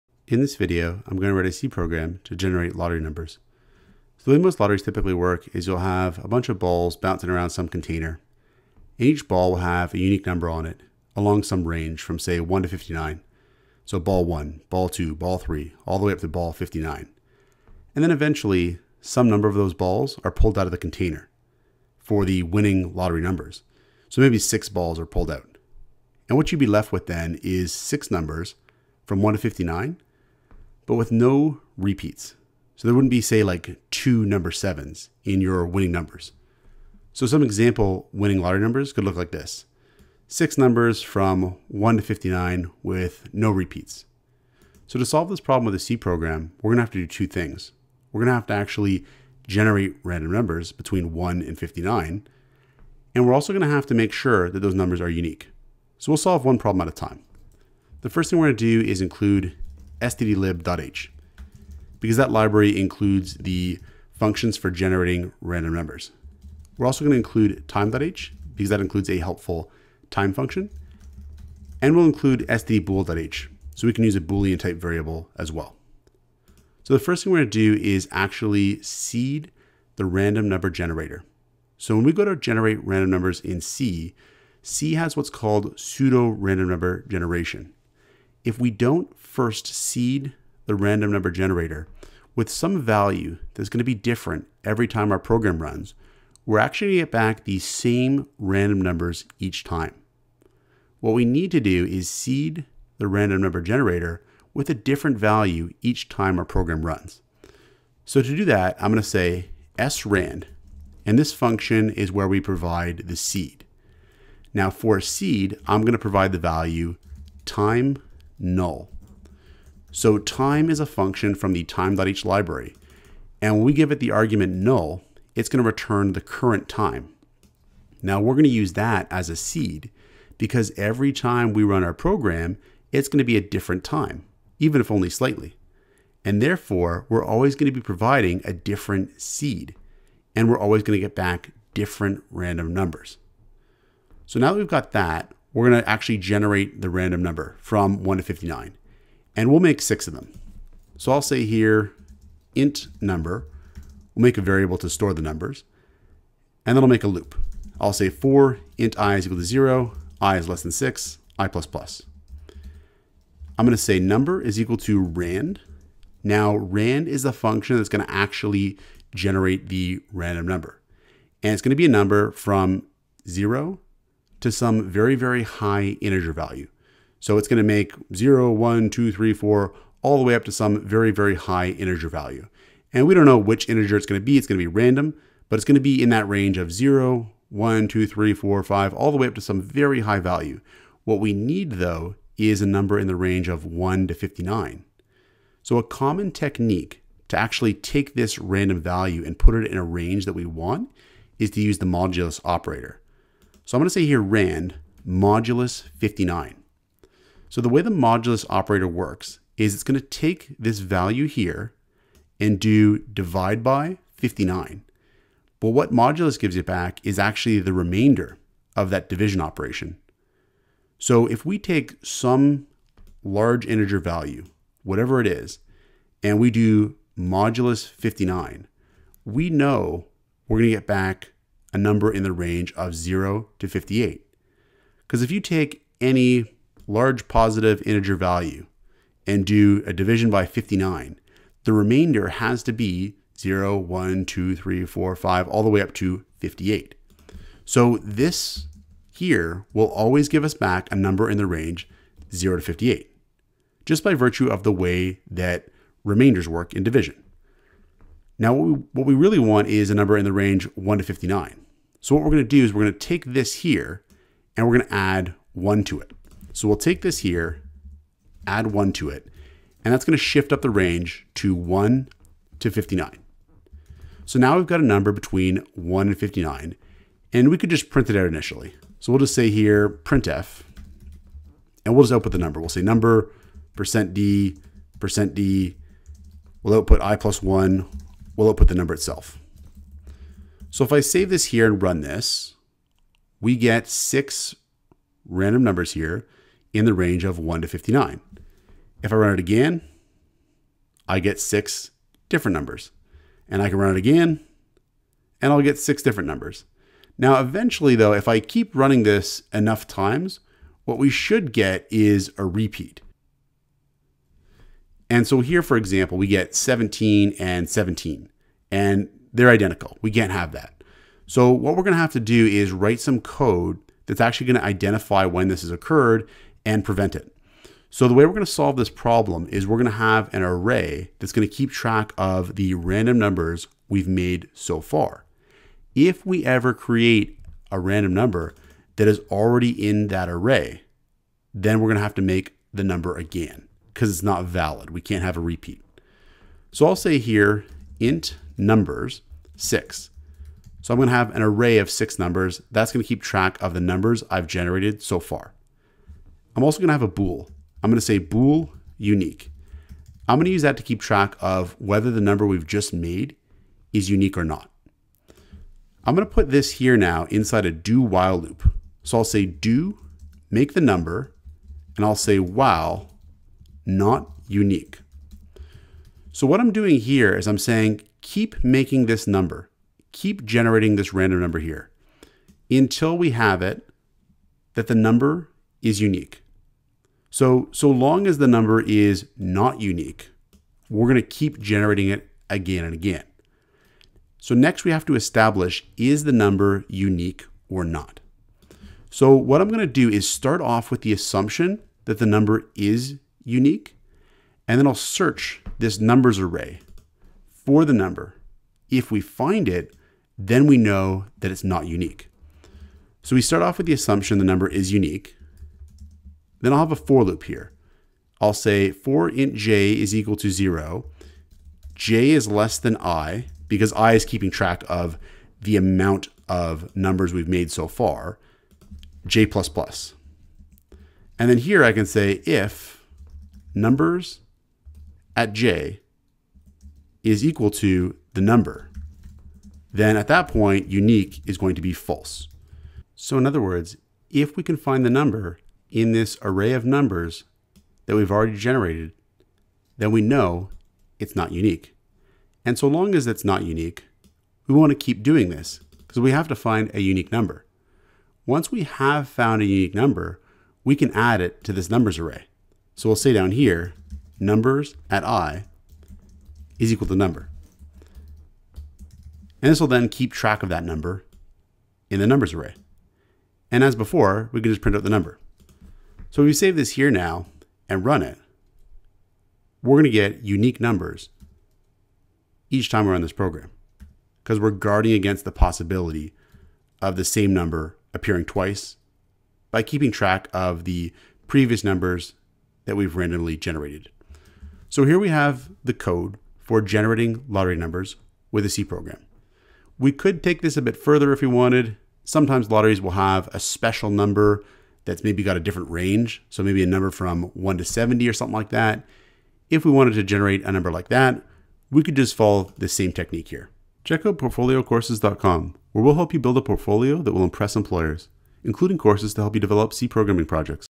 Lottery Number Generator | C Programming Example
Example of generating random lottery numbers using C. Source code to build a portfolio that will impress employers!